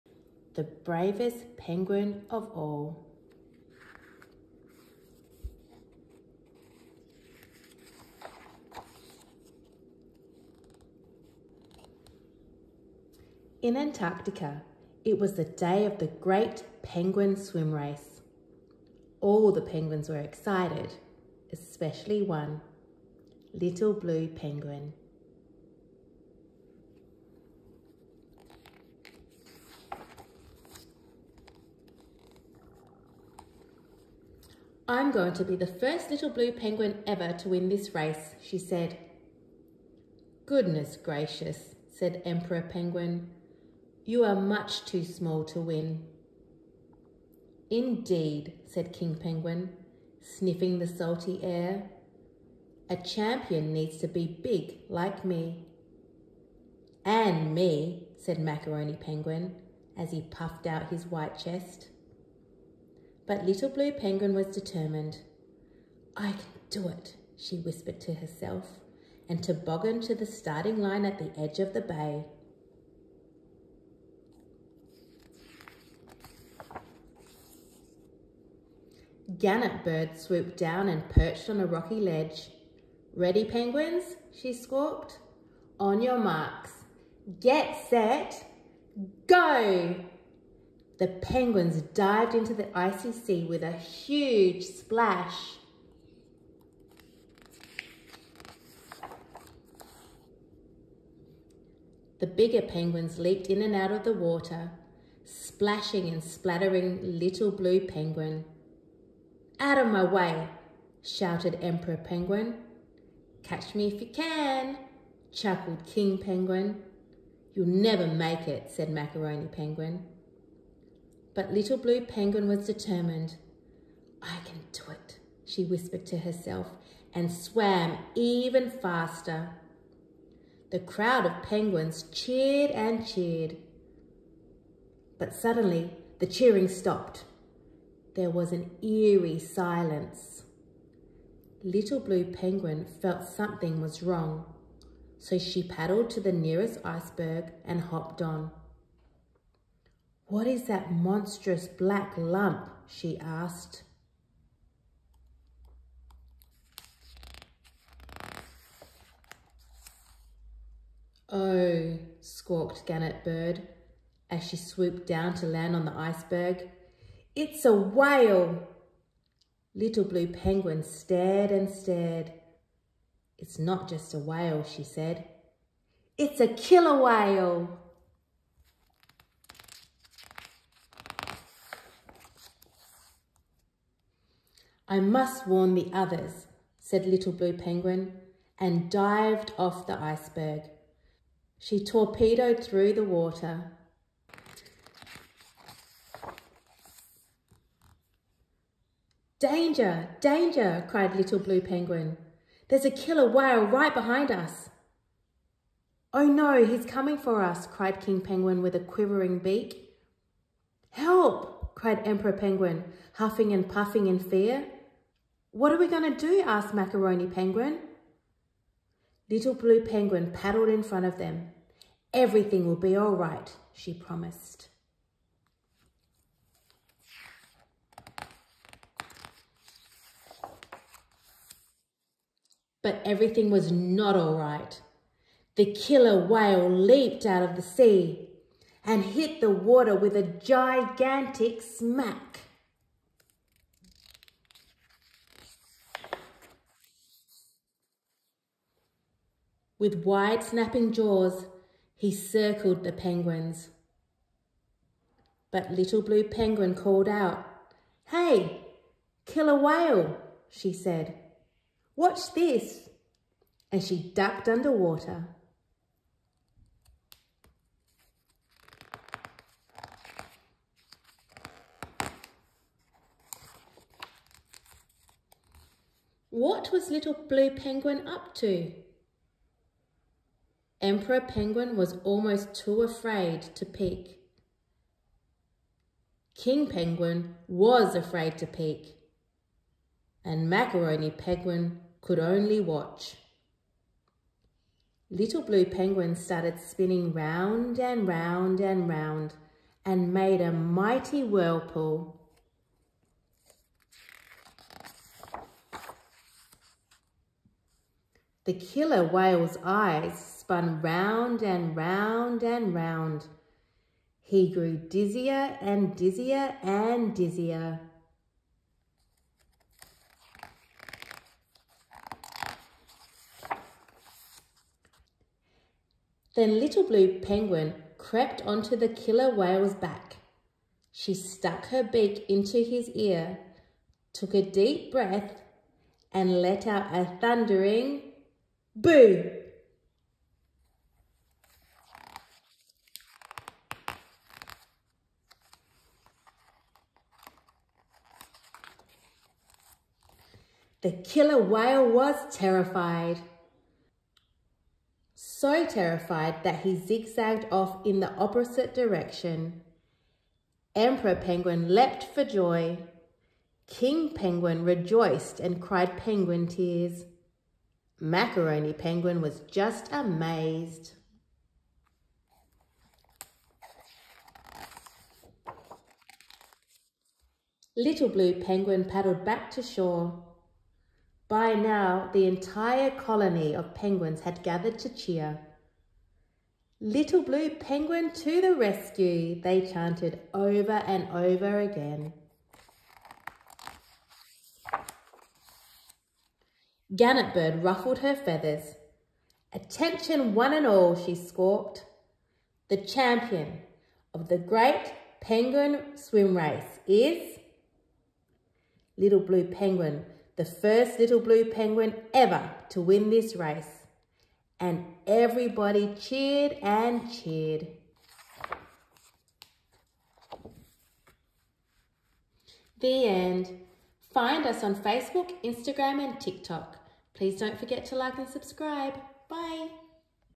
THE BRAVEST PENGUIN OF ALL by Rina A. Foti | Educator Jo Read Aloud Kids Books
In this video, Educator Jo reads the children's book 'The Bravest Penguin of All' by author Rina A. Foti and illustrated by Cathy Abadie.

Little Blue Penguin wants to win the race, but the other penguins think he's too small. But Little Penguin is not too small to save them from the killer whale! When danger threatens, only Little Blue Penguin is clever and brave enough to save the others.

Where to buy this book...